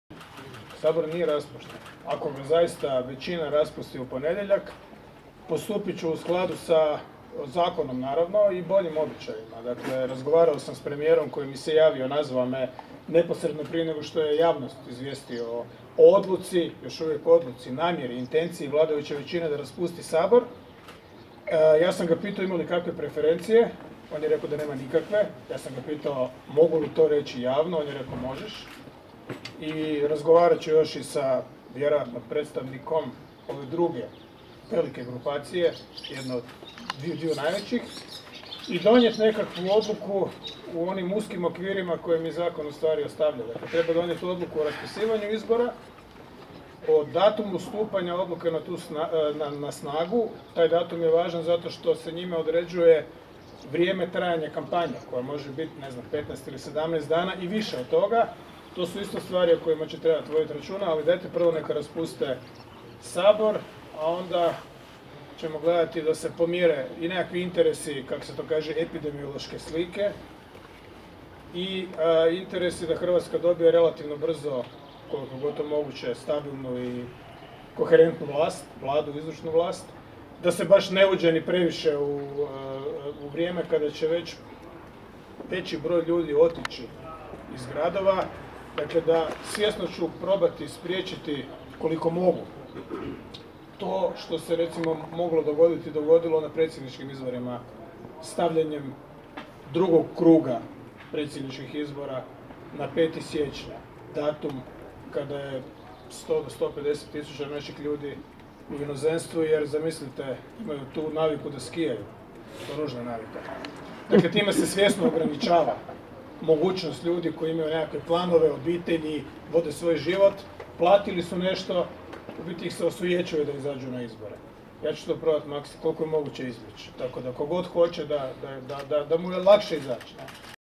Milanović: Spriječit ću da se izbori održe kada je dosta Hrvata u inozemstvu
Ptuj, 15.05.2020 - Hrvatski predsjednik Zoran Milanović poručio je u petak iz Maribora da će raspisati izbore i postupiti u skladu sa zakonom kad se u ponedjeljak raspusti Sabor, ali i naglasio kako će probati spriječiti da se izbori održe u vrijeme kada je dosta Hrvata u inozemstvu.